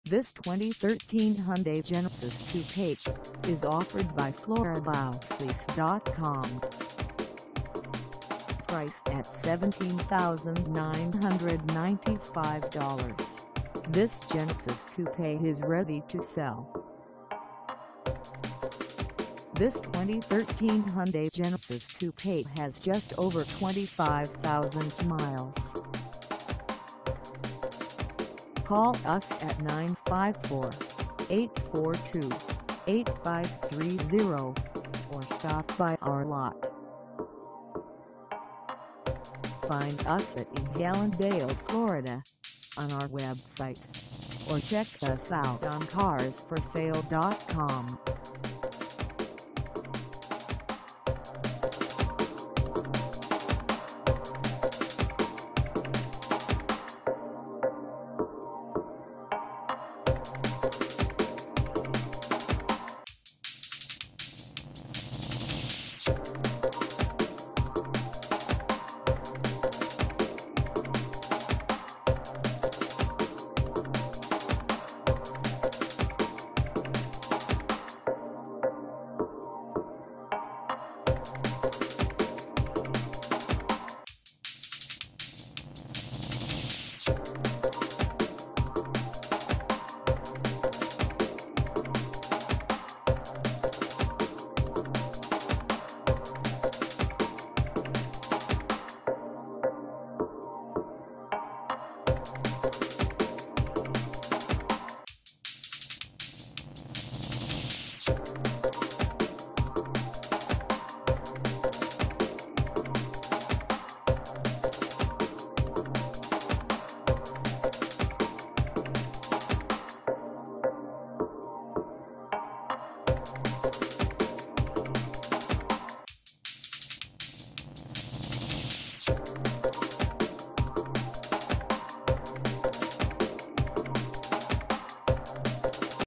2013 Hyundai Genesis Coupe 2.0T 2dr Coupe 6M for sale in hal
This 2013 Hyundai Genesis Coupe 2.0T 2dr Coupe 6M is for sale in hallandale, FL 33009 at Floridaofflease.com.
Engine: I4 2.0L I4 Turbo
Transmission: Manual 6-Speed
Color: Red
Mileage: 25000
Contact Floridaofflease.com  hallandale, FL 33009 to test drive this 2013 Hyundai Genesis Coupe 2.0T 2dr Coupe 6M today.